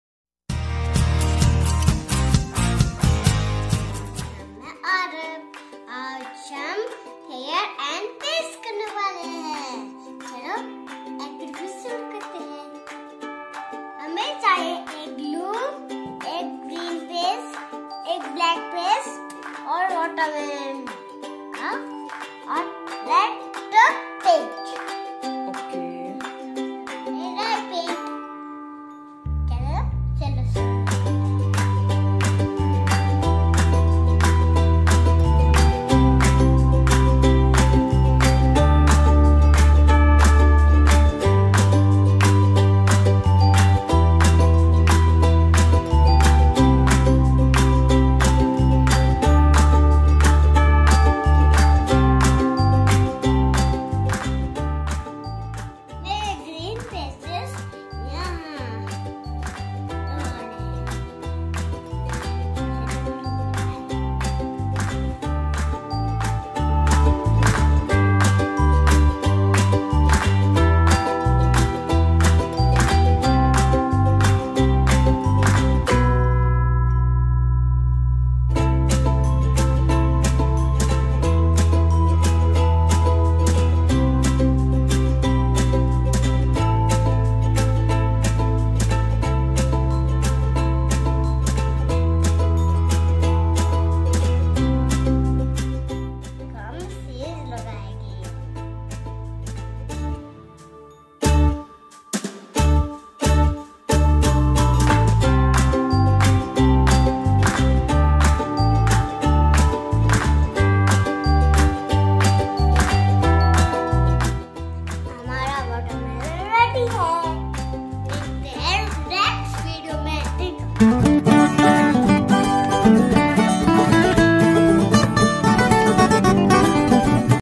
|Paper Tearing and Pasting| Paper Craft |fruit (watermelon)| Kids deeds | Aarav
hello everyone !!
today's video is all about tear and pasting activity for kids .
this is a very interesting activity to do with your kids at home .